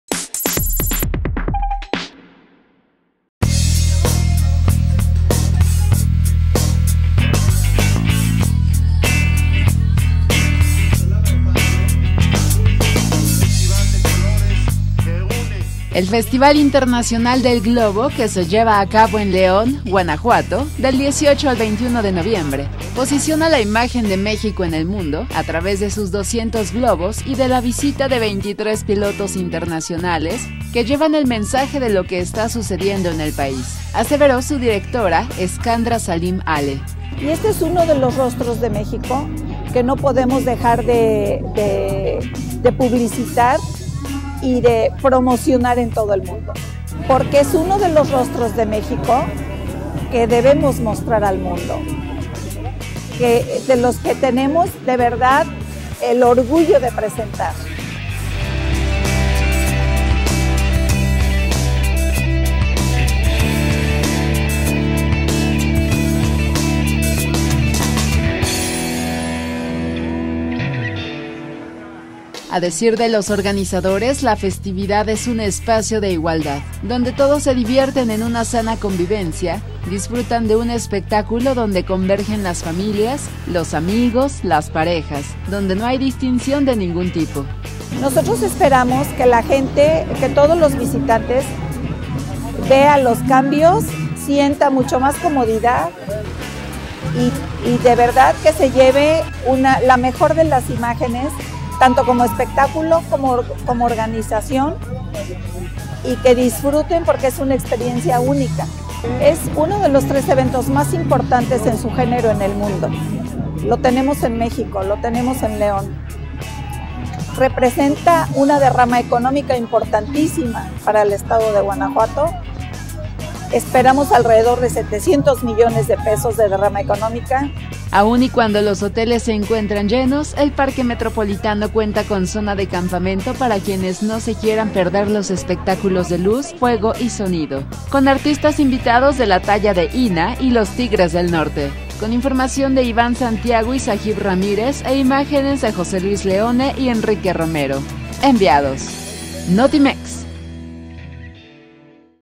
Festival del Globo posiciona la imagen de México en el mundo
El Festival Internacional del Globo de León, Guanajuato, posiciona la imagen de México en el mundo, a través de la visita de 23 pilotos que llevan el mensaje de lo que aquí está sucediendo, aseveró su directora, Escandra Salim Alle